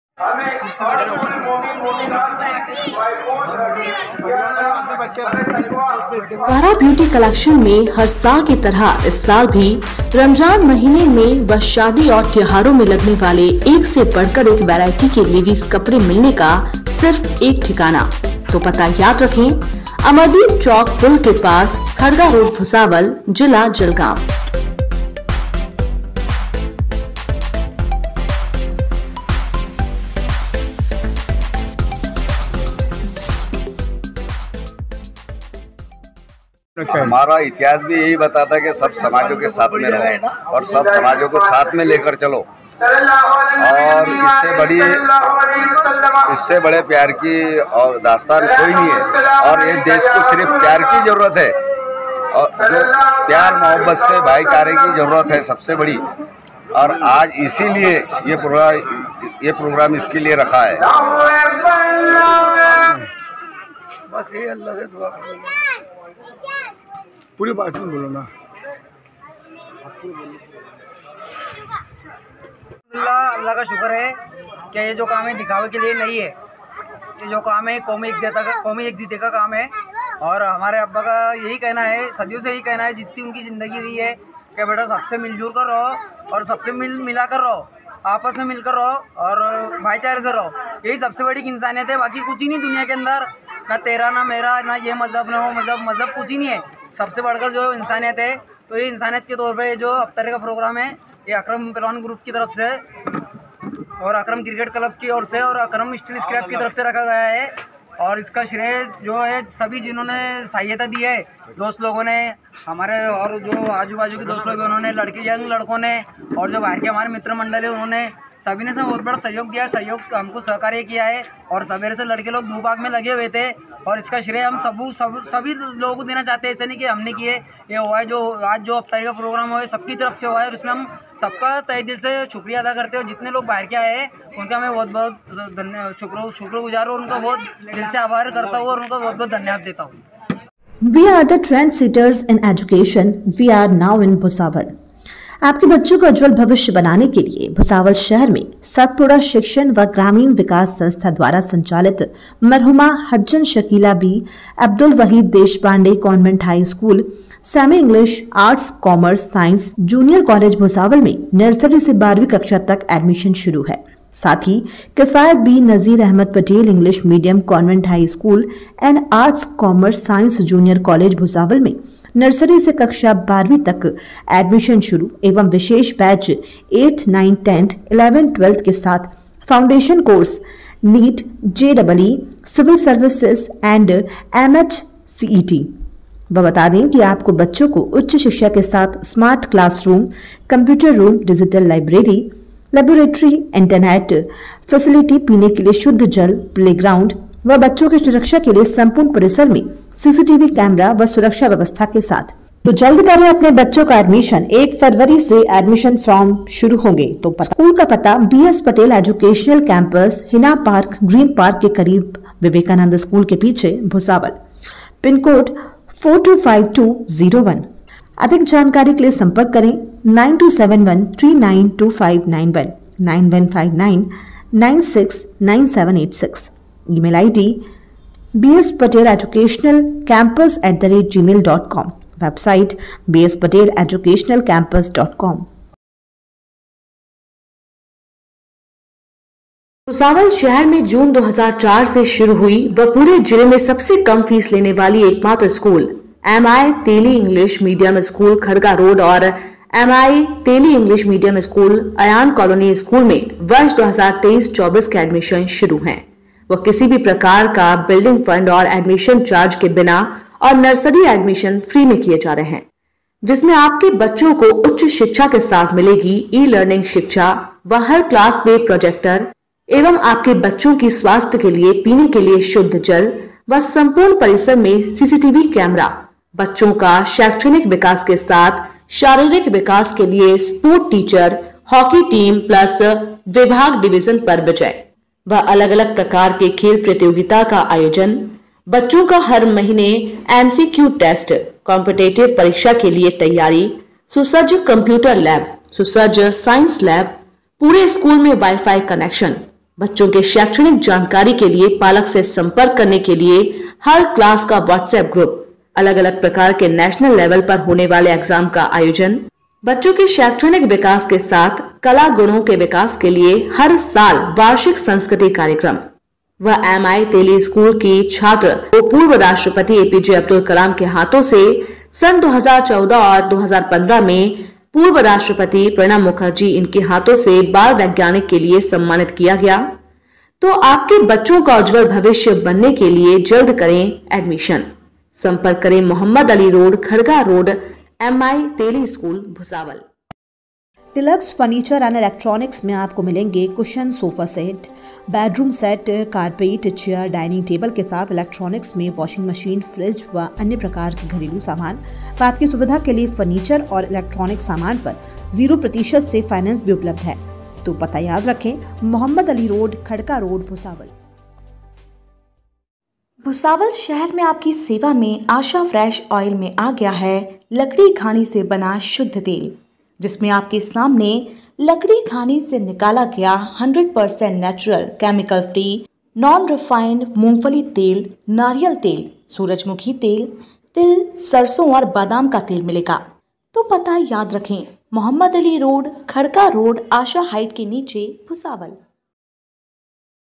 भुसावल : शेख परिवार की ओर से पंचशील नगर में इफ्तार पार्टी का आयोजन.! | BK Times न्यूज़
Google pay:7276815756